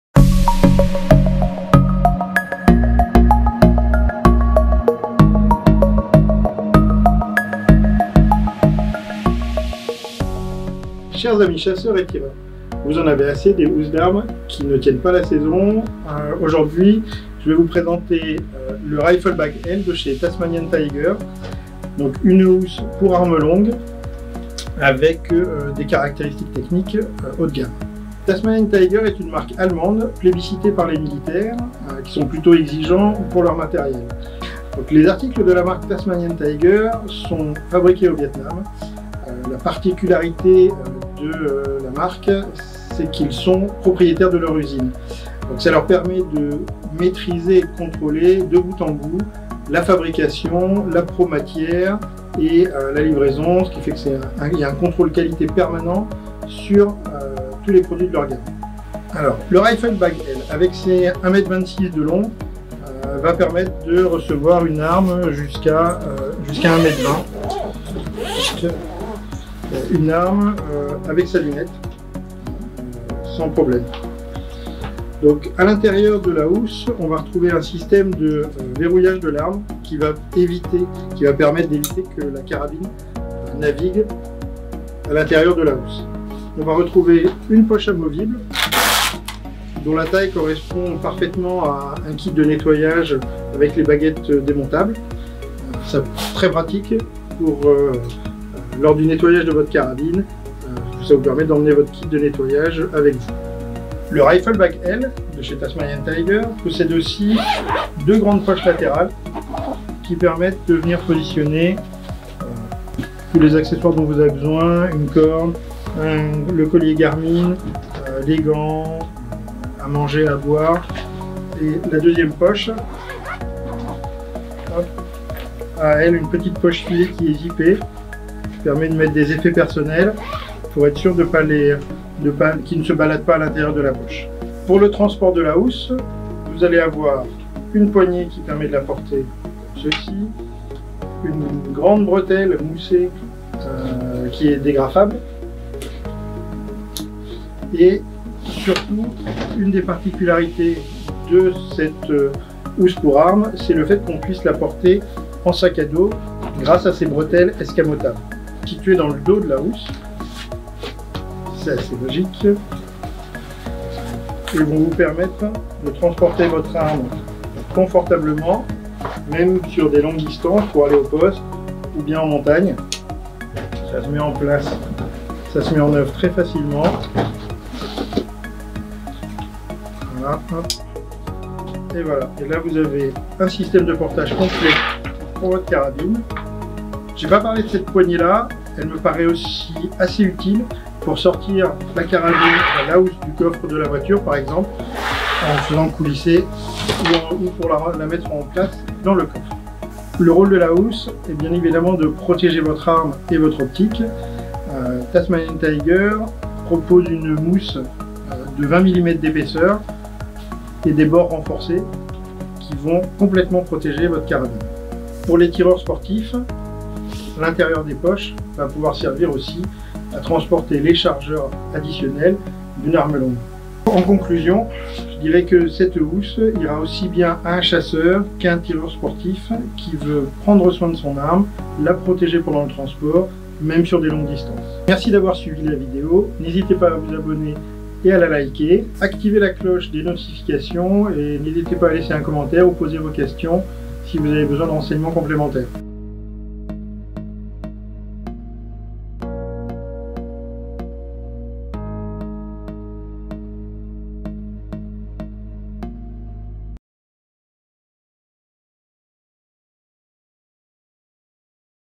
Protégez parfaitement votre arme - Rifle Bag L
Avec le Rifle Bag L de Tismanian Tiger, vous êtes assurés de protéger votre arme en toute circonstances, même dans des environnements exigeants.